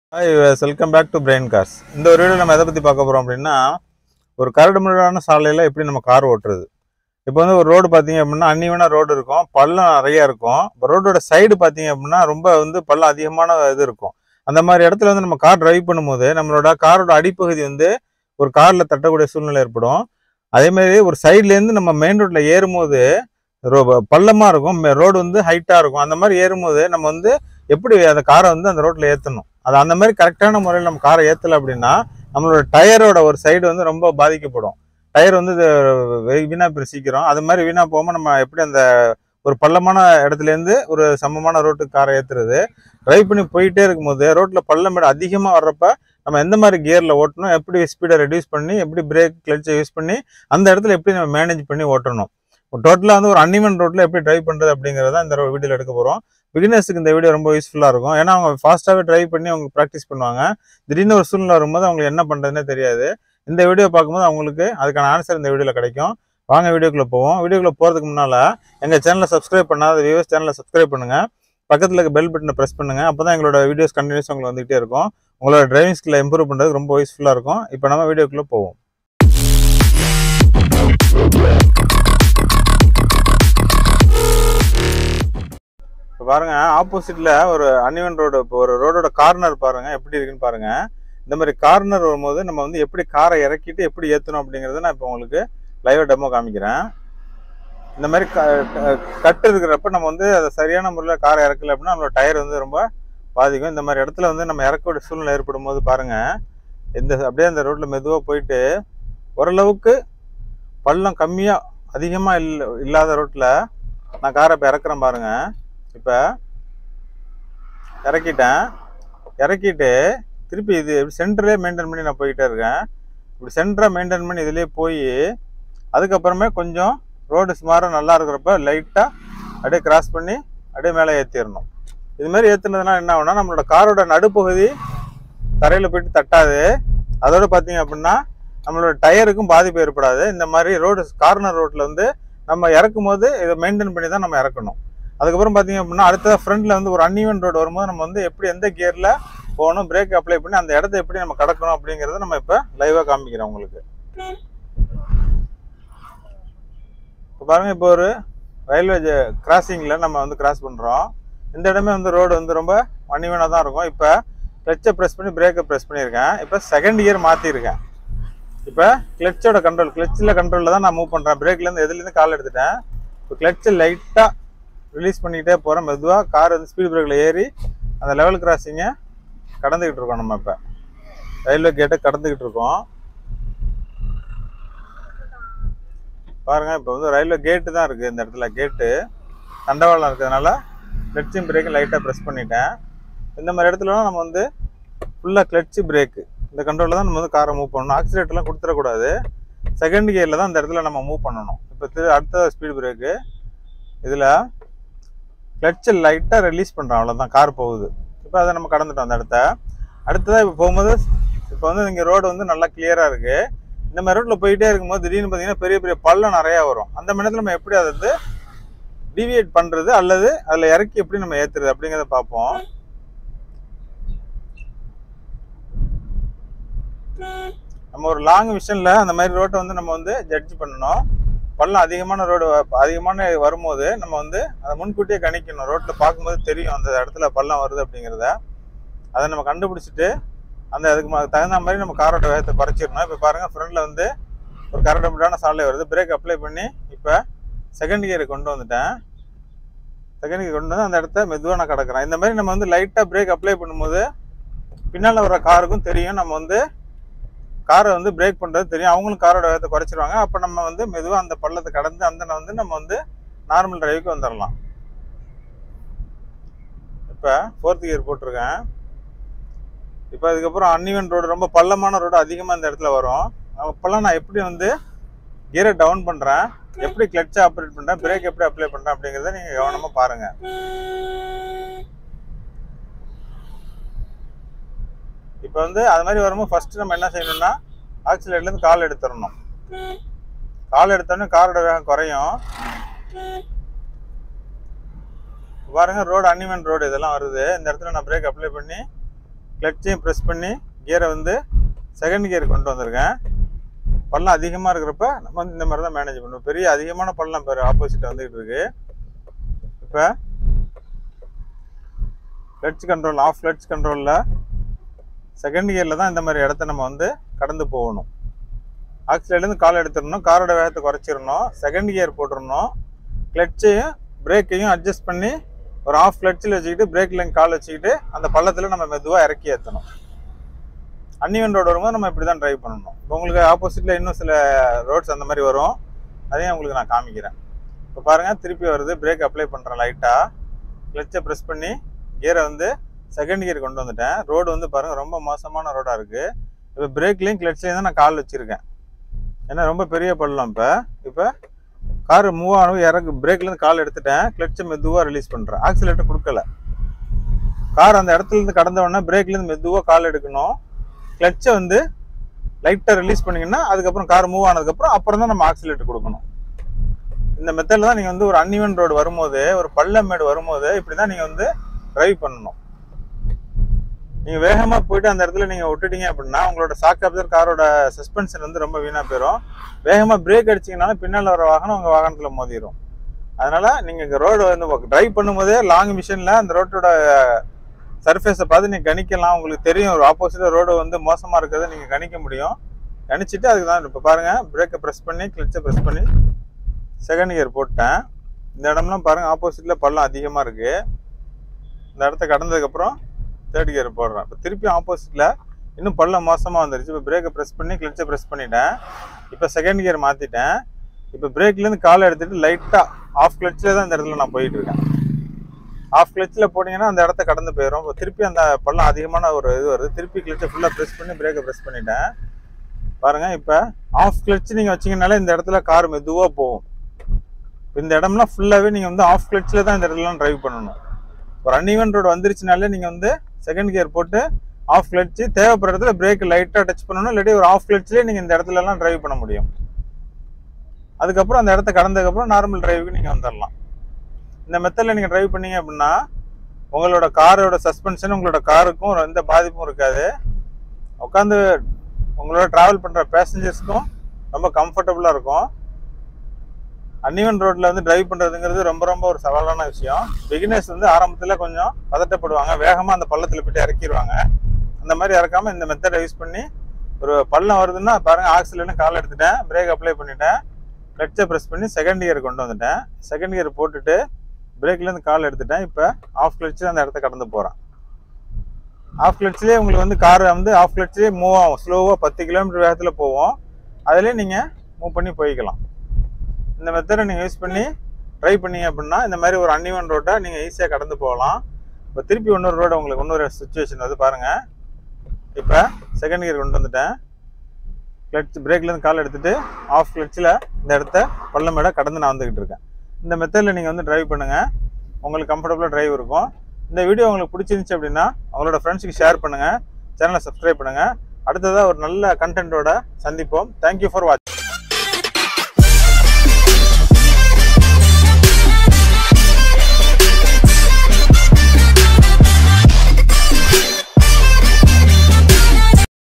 beginers how to use clutch and break on slow driving @brain cars
This video explain beginers how to drive on uneven roads with half clutch and break controls@brain cars